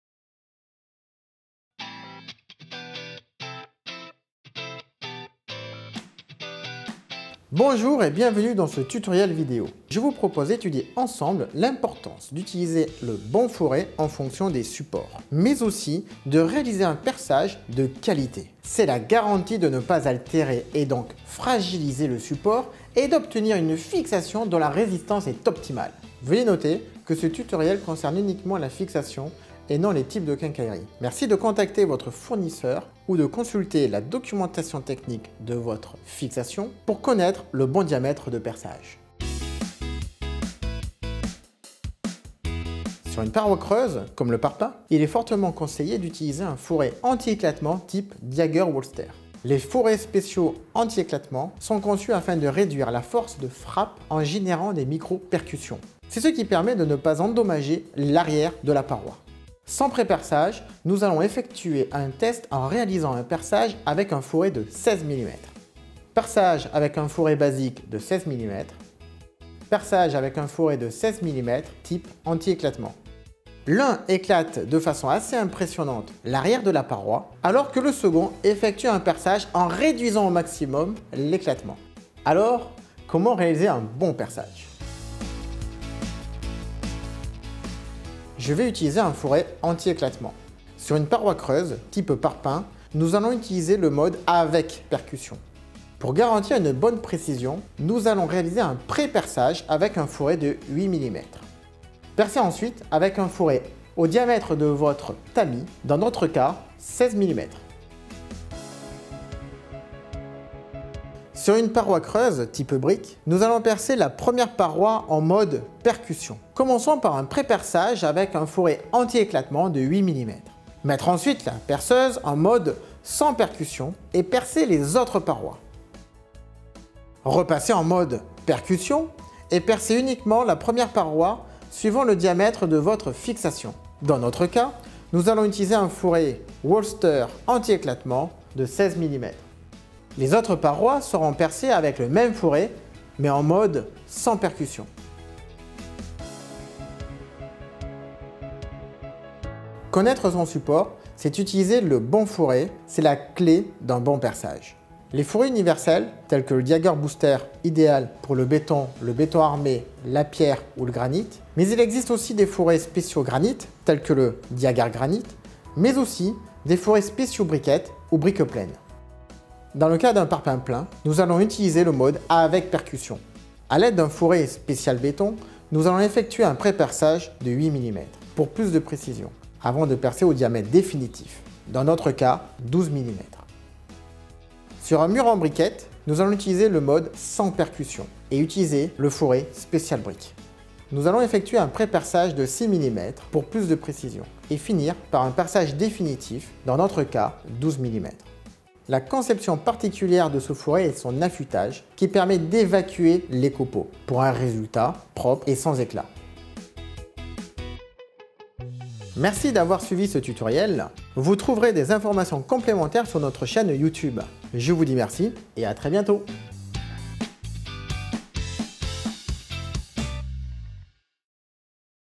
Tuto percage FRANCIAFLEX
Cette vidéo vous présente les bons gestes pour réaliser un bon perçage.
Pour information, vous trouverez ci dessous quelques liens pour plus de renseignements.